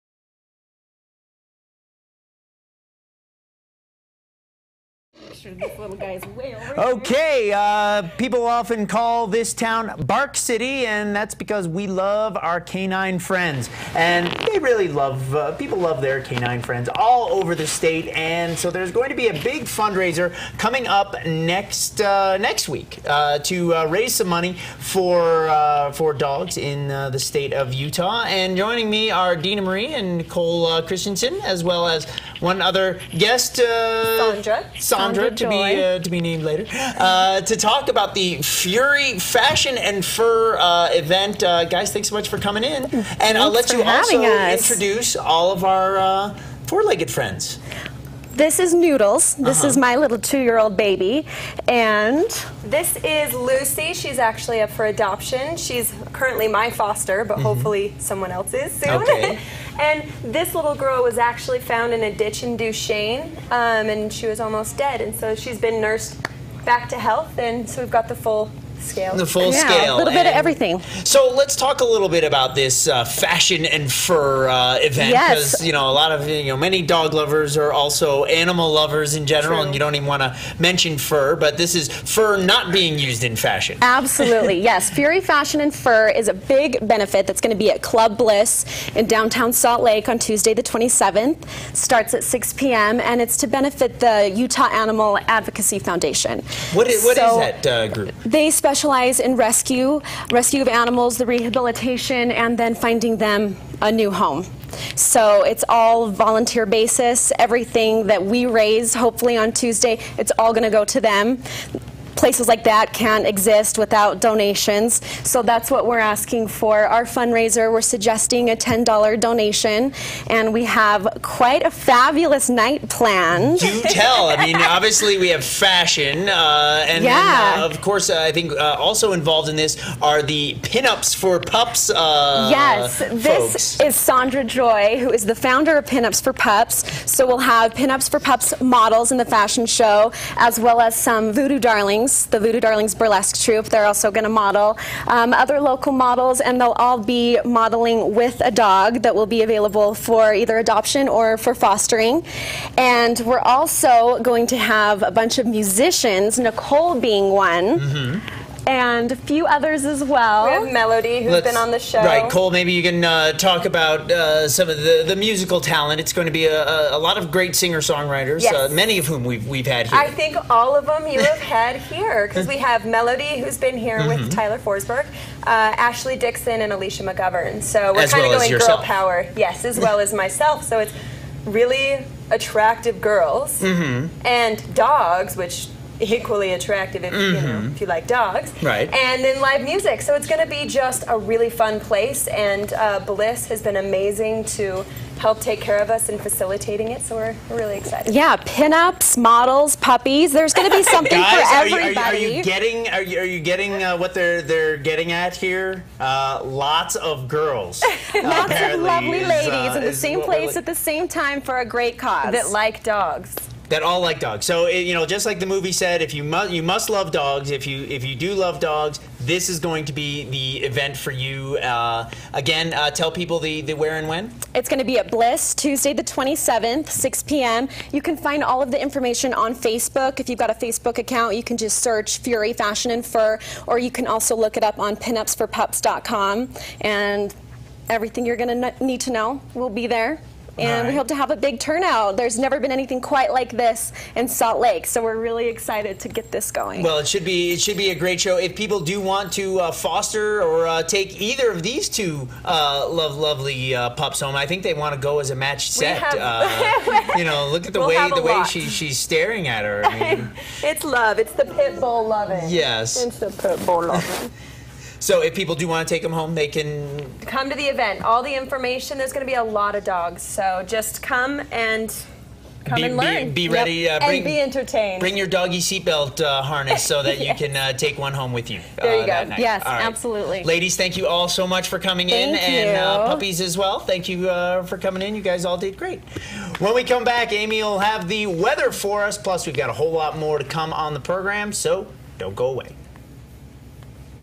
Pinups for Pups presents Fury, Fashion and Fur!
Join Pinups for Pups, Voodoo Darlings, and host Deena Marie for a fashion show, burlesque performance and live music by Kole, Melody, ashlee dickson and alicia Mcgovern.